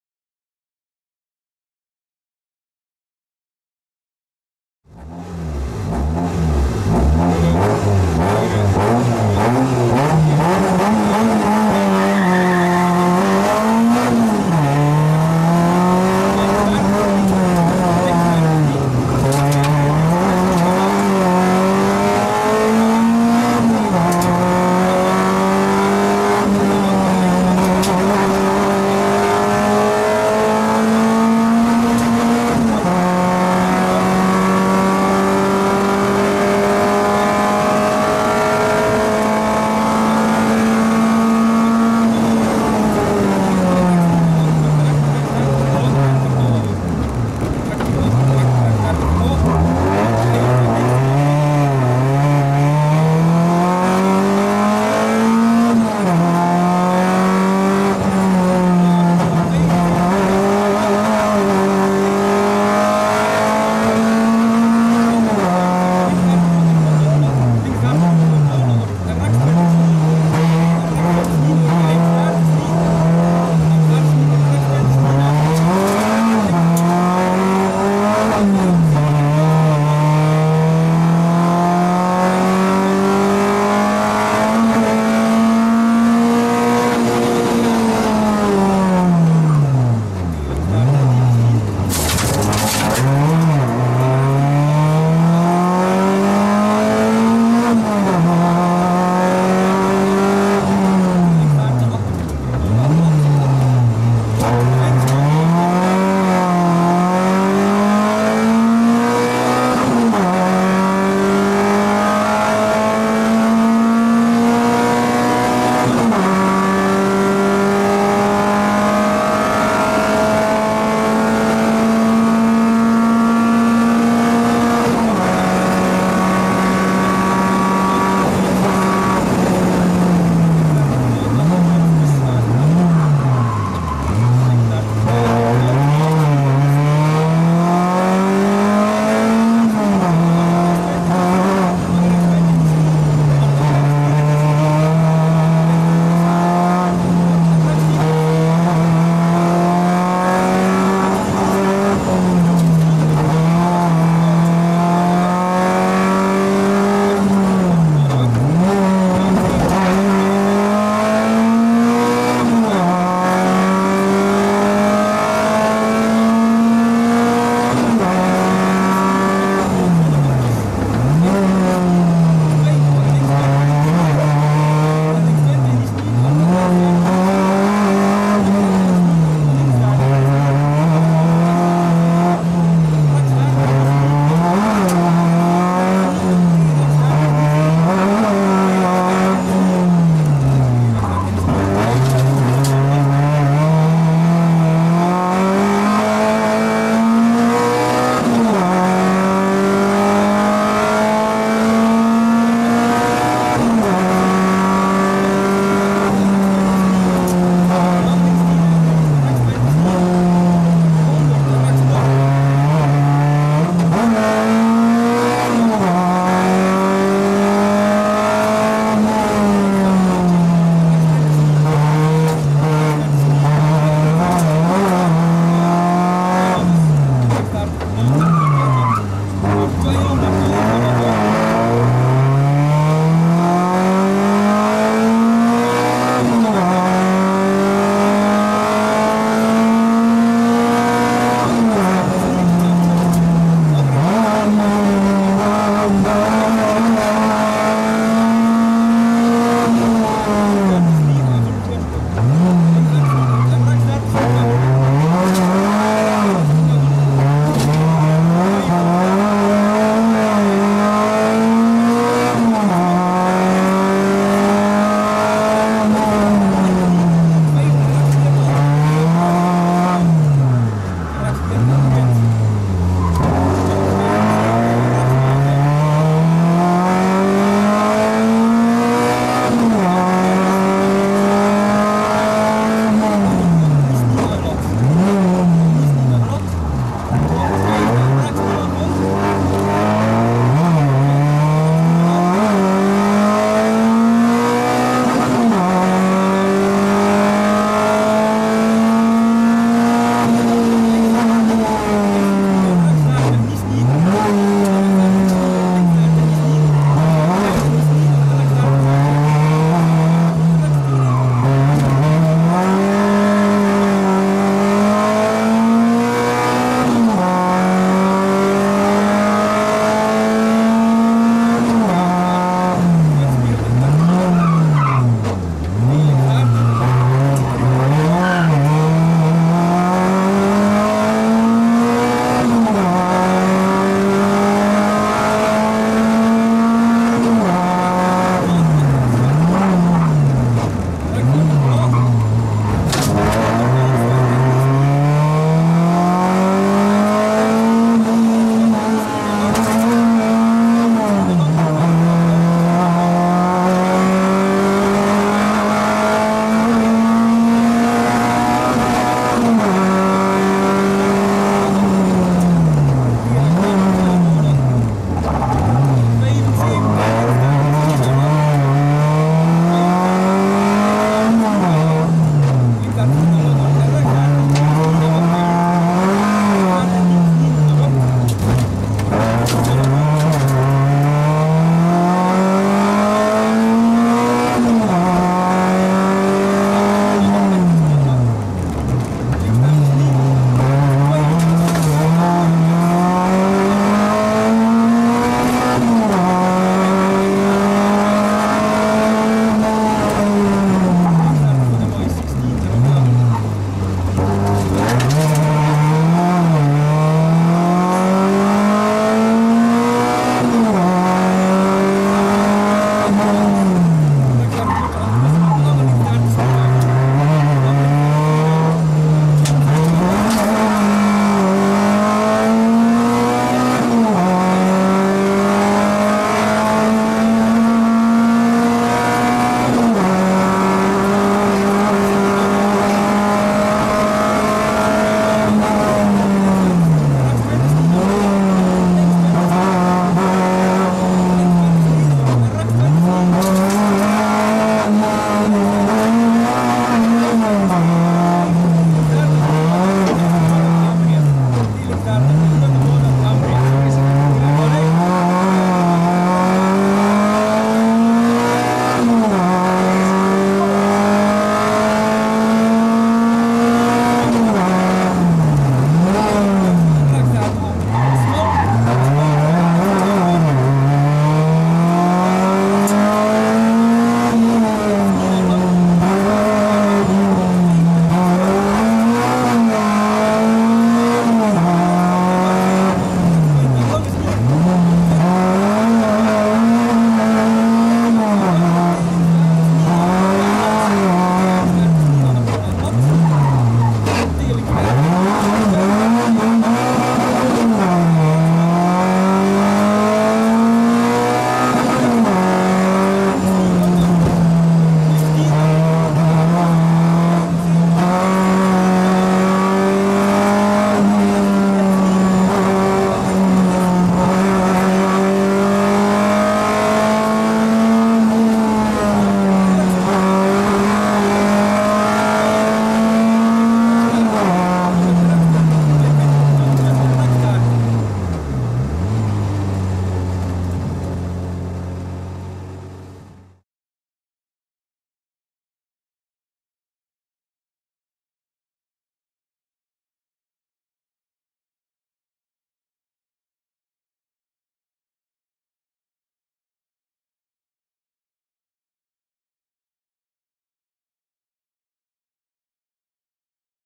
Onboard | S. Blanchard - J. Naessens | SS7 Kortemark | Hemicuda Rally 2021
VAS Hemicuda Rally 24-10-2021
SS 7 Kortemark
Onboard: Stijn Blanchard - Jens Naessens
Citroën Saxo